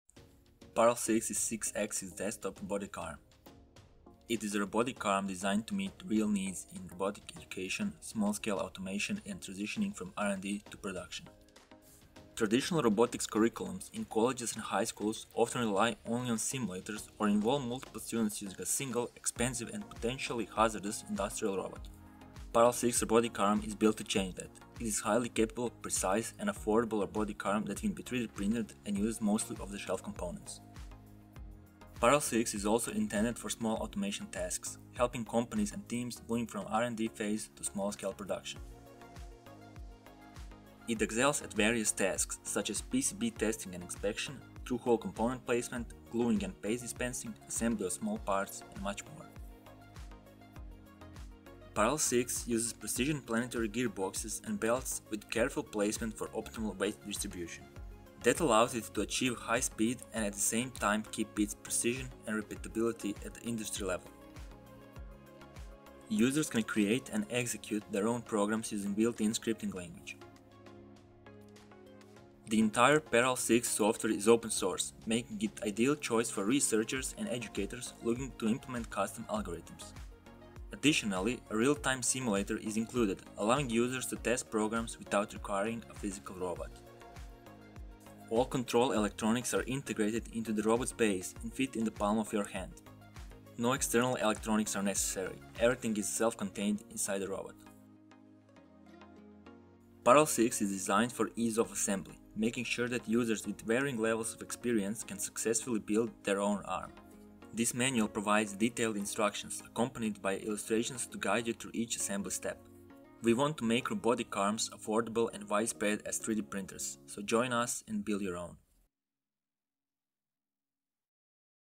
PAROL6 - 3D printed desktop robot arm
PAROL6 is a high-performance 3D-printed desktop robotic arm. The design approach of PAROL6 was similar to industrial robots in terms of mechanical design, control software, and usability. Control software, GUI, and robots STL files are open-source.